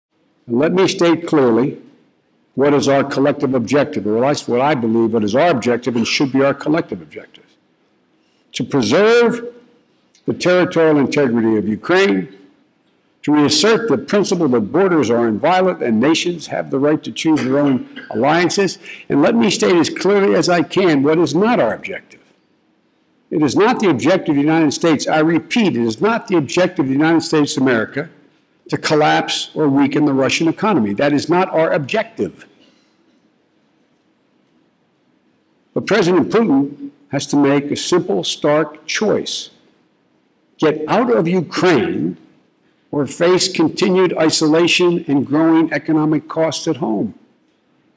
Our Collective Objective: Preserving the Territorial Integrity of Ukraine
Vice President of the United States Joe Biden delivered remarks at the Munich Security Conference in Munich, Germany on Saturday, February 7, 2015.